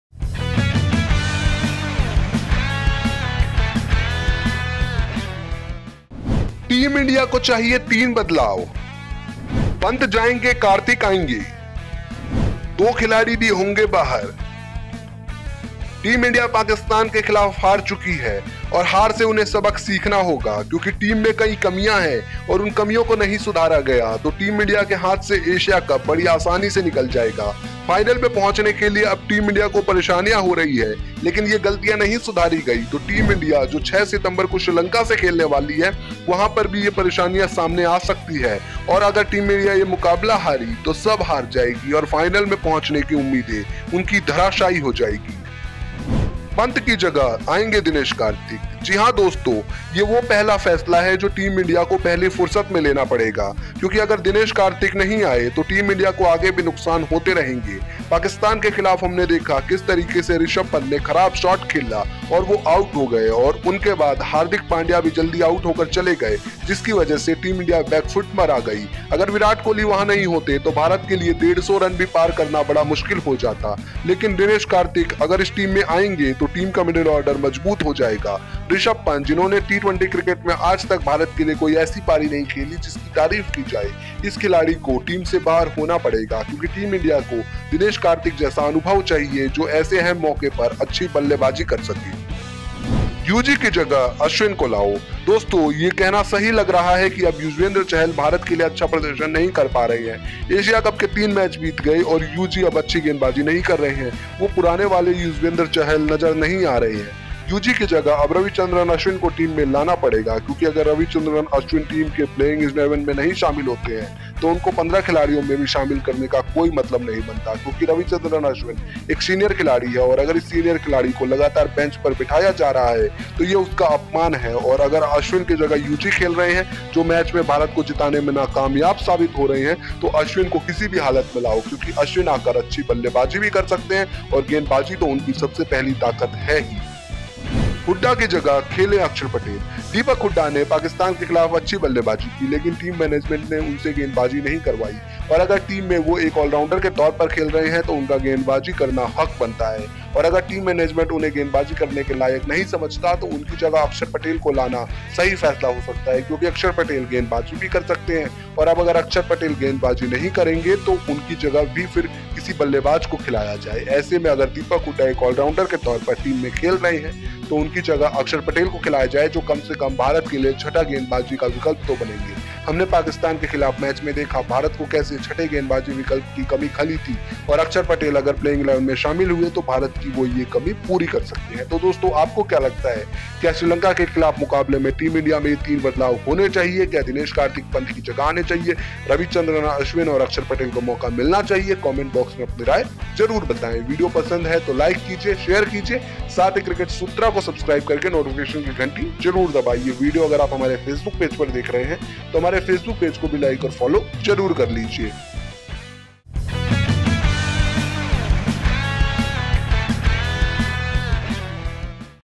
Ind vs SL : Team India करेगी 3 बदलाव, Rishabh Pant जाएंगे DK आएंगे, 2 धांशू खिलाड़ी भी होंगे बाहर
Ind vs SL Asia Cup 2022: Team India करेगी 3 बदलाव, Rishabh Pant जाएंगे DK आएंगे, 2 धांशू खिलाड़ी भी होंगे बाहर

* Cricket News
* Match Schedule
* Players Interviews
* Live Sessions
* IPL News
* BCCI News
* ICC Tournaments
* Depth analysis